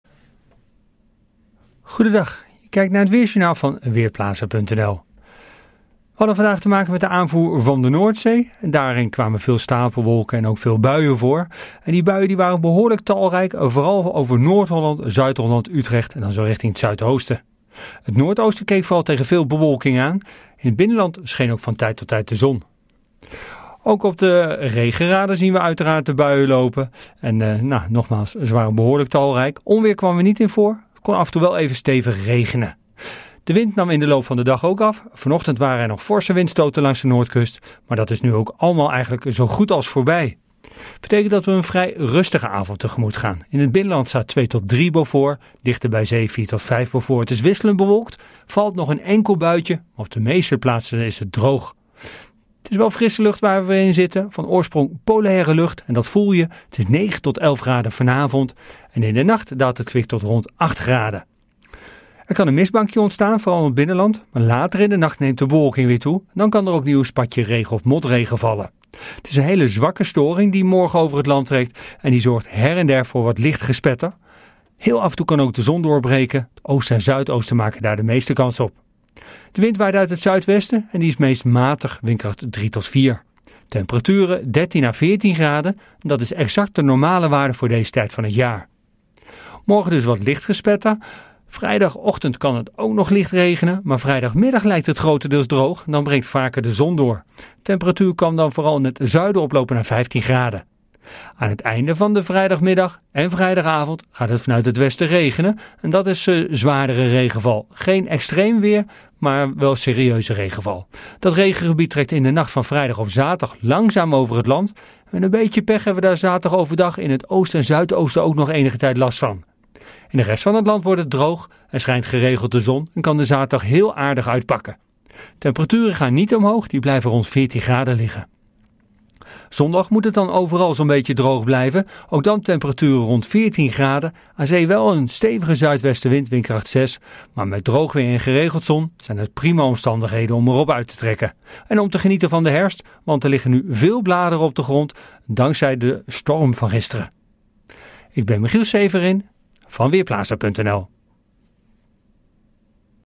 Weerbericht woensdagmiddag: Licht wisselvallig
Zowel de wind als de buiigheid nemen de komende uren af. Vannacht is het vrijwel overal droog en bij weinig wind kan zich lokaal een mistbank vormen. Het koelt af tot 8 graden. In de nanacht en morgen overdag hebben wolken de overhand en valt er af en toe motregen of lichte regen. De zon breekt slechts nu en dan door en 's middags is het 13 of 14 graden. Vrijdag valt er eerst nog wat regen, maar al snel wordt het droog en breekt de zon door. Tegen de avond gaat het vanuit het westen weer regenen.